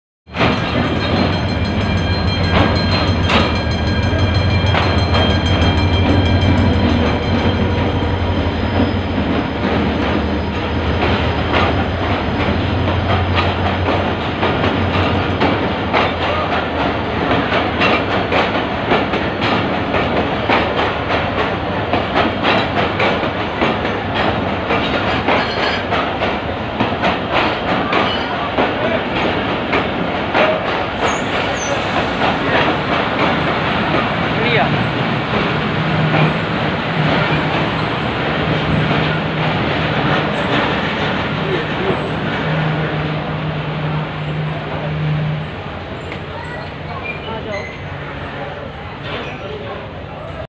151UP Shah Latif Express l Karachi cantt l Pakistan Railways
151UP shah Latif Express Train at 07:25 HRS karachi cantt station